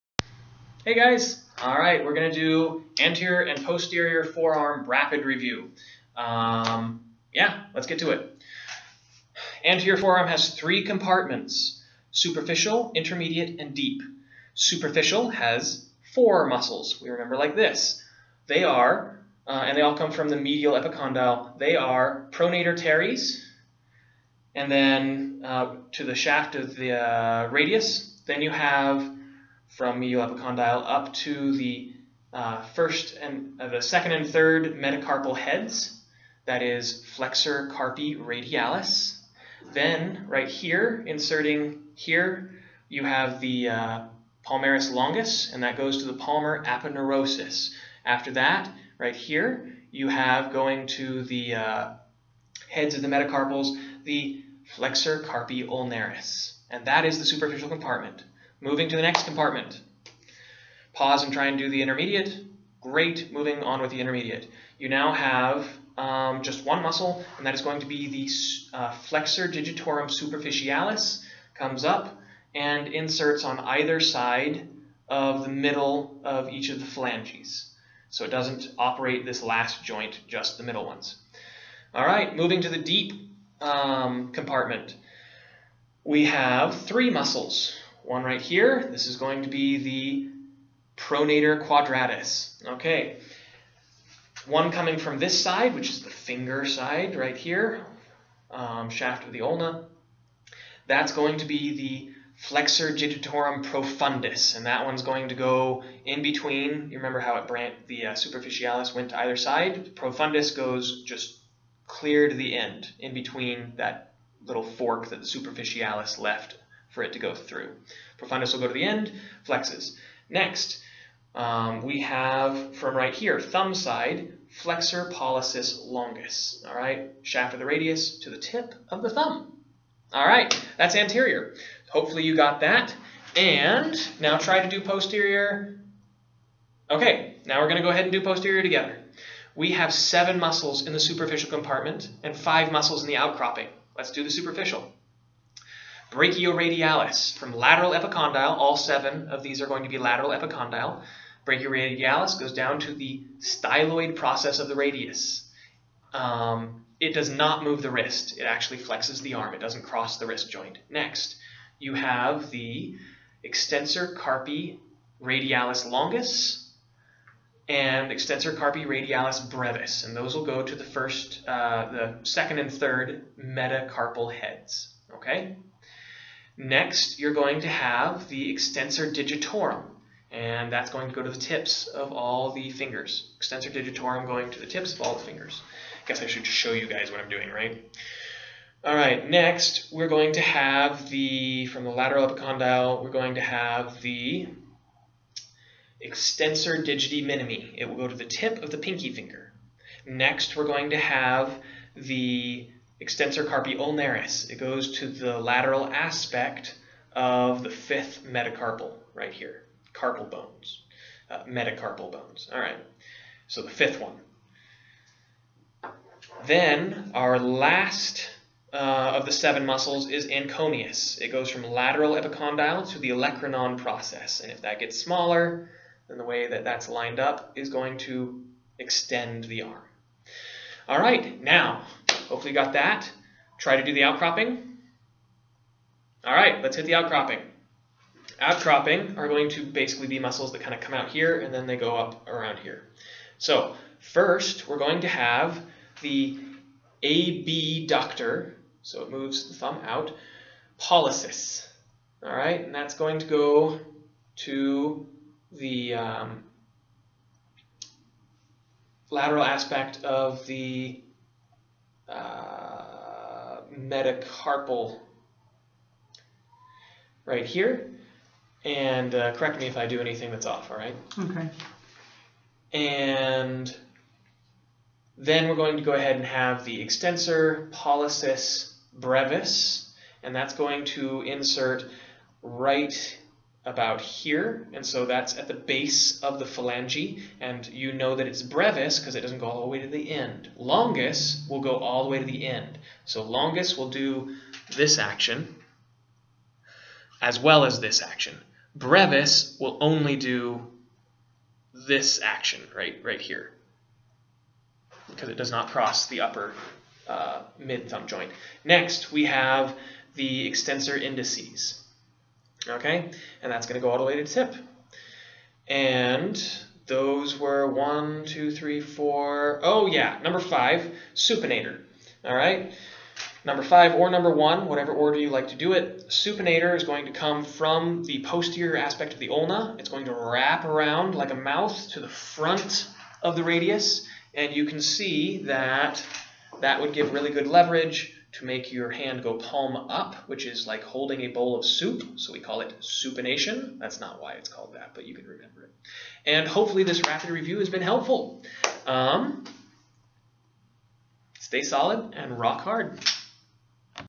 Anatomy Q1 - Rapid Review - Muscles of the Anterior and Posterior Forearm
Want it to stick? Test yourself (aka practice active recall!) in between each chunk ;)

Stay solid my friends!